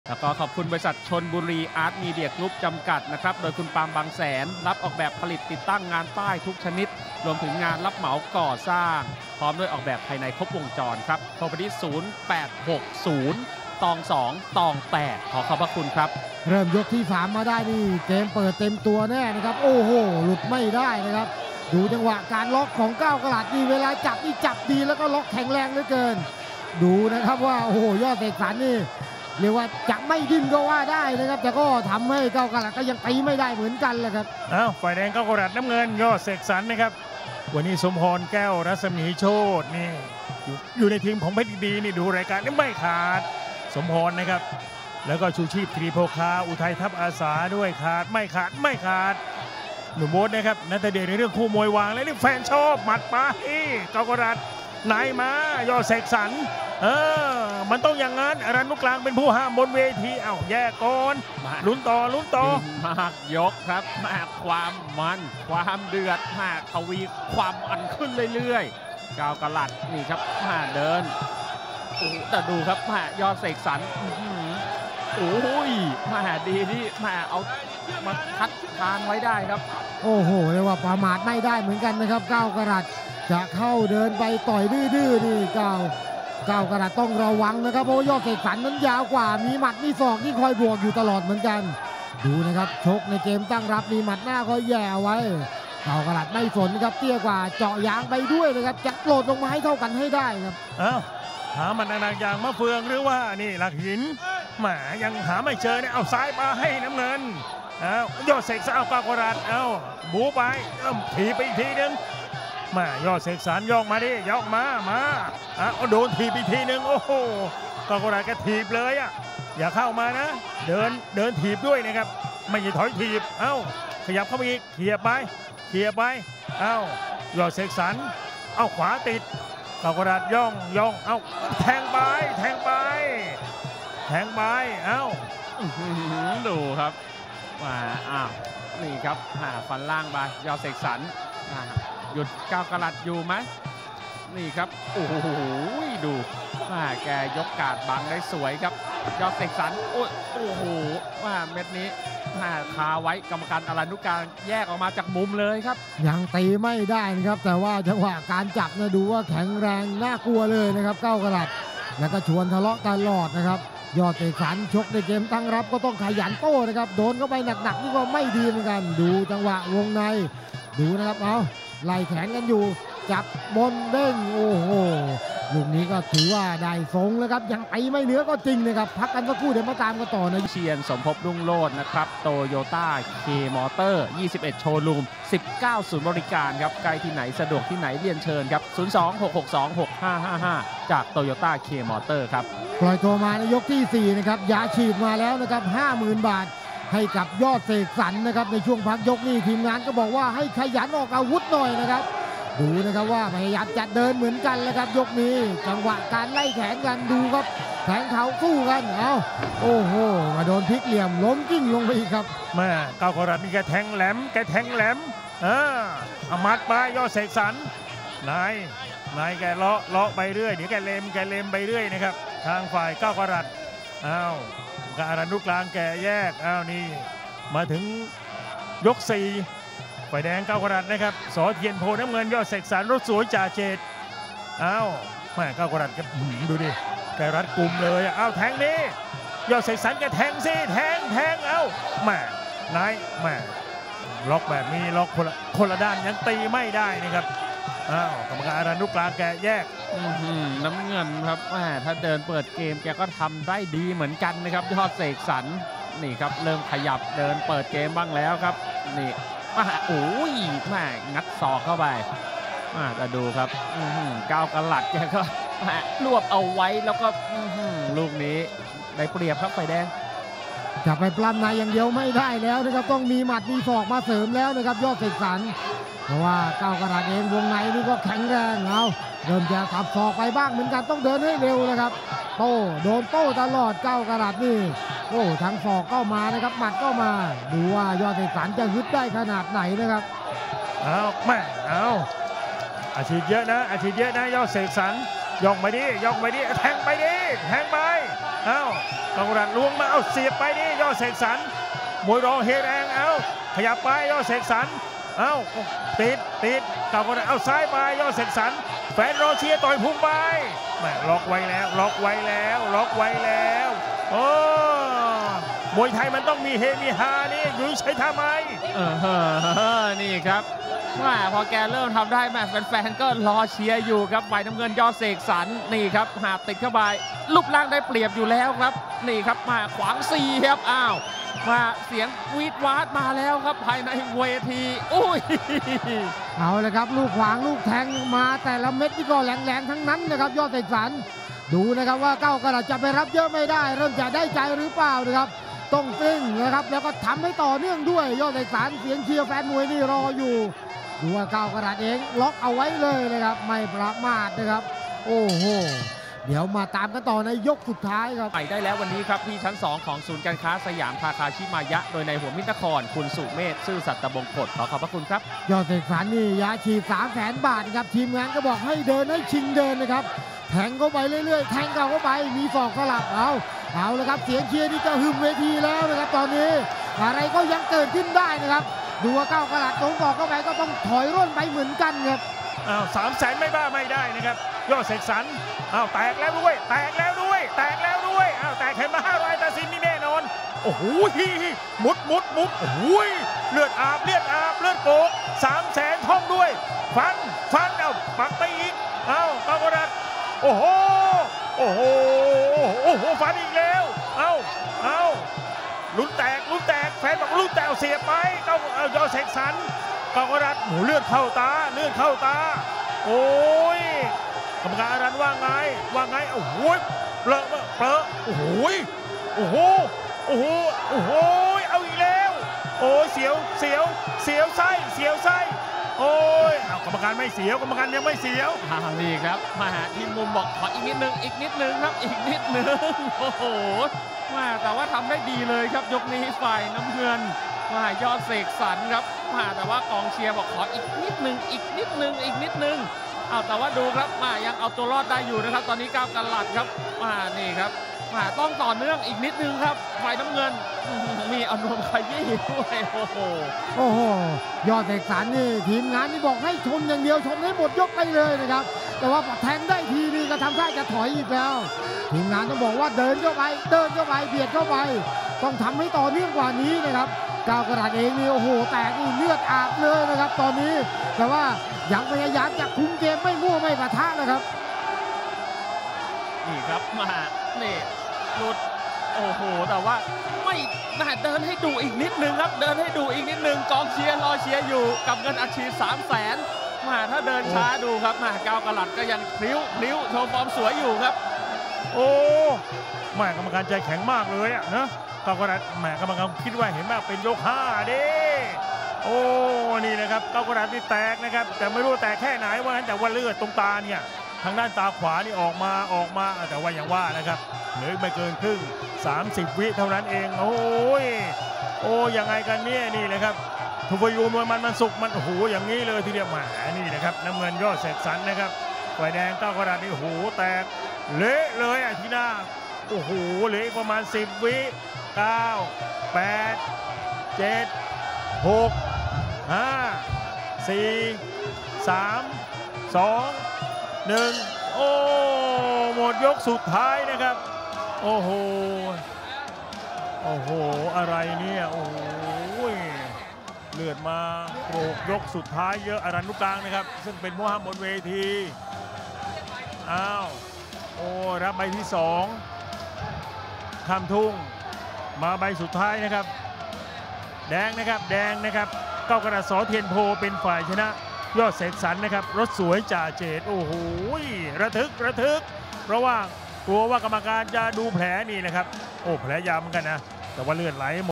เก้ากระรัต VS ยอดเสกสรร | KAOKARAT VS YODSEKSAN | มวยมันส์วันศุกร์
เก้ากระรัต VS ยอดเสกสรร | KAOKARAT VS YODSEKSAN | มวยมันส์วันศุกร์ วันศุกร์ ที่ 16 มิถุนายน 2566 เก้ากระรัต ส.เทียนโพธิ์ VS ยอดเสกสรร รถสวยจ่าเจตสายพริ้ว Muaymunwunsuk Fight between KAOKARAT SOR.TAINPOH VS YODSEKSAN RODSUAYJAJETSAIPRIW 16 June , 2023 Muaymunwunsuk, Rangsit Stadium.